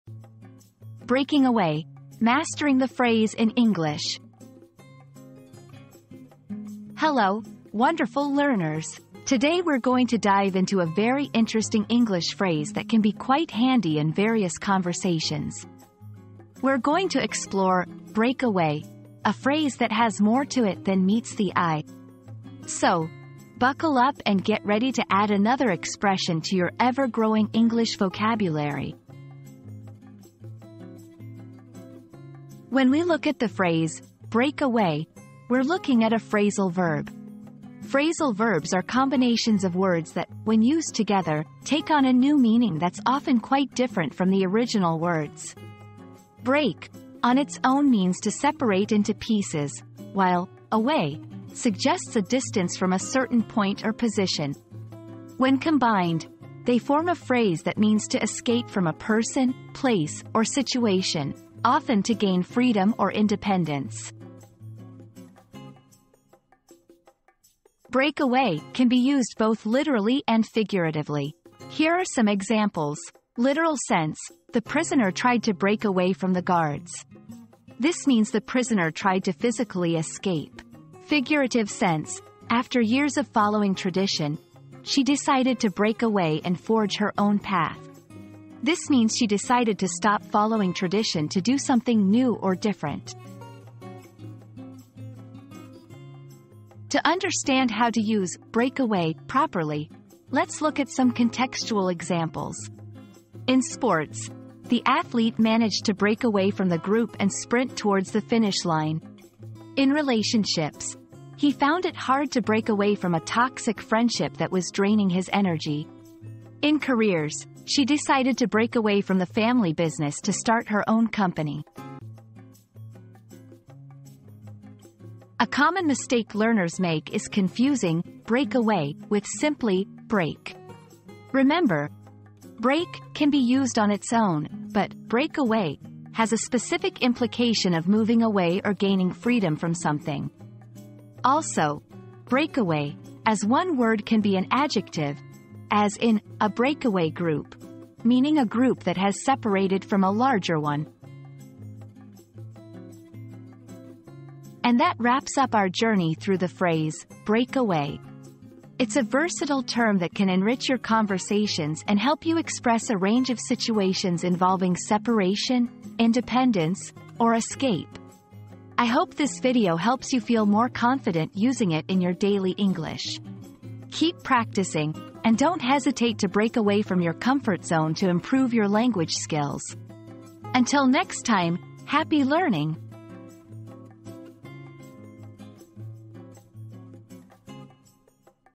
Breaking Away: Mastering the Phrase in English
Breaking Away: Mastering the Phrase in English • Learn how to master the phrase 'break away' in English and add it to your vocabulary. Explore the literal and figurative meanings of this phrasal verb and discover how to use it in different contexts. Whether you want to escape a situation or forge your own path, 'break away' is a versatile expression that can be quite handy in conversations.

00:00 • Introduction - Breaking Away: Mastering the Phrase in English
00:32 • Understanding "Break Away"
01:07 • Usage of "Break Away"
01:36 • Contextual Examples
02:04 • Common Mistakes to Avoid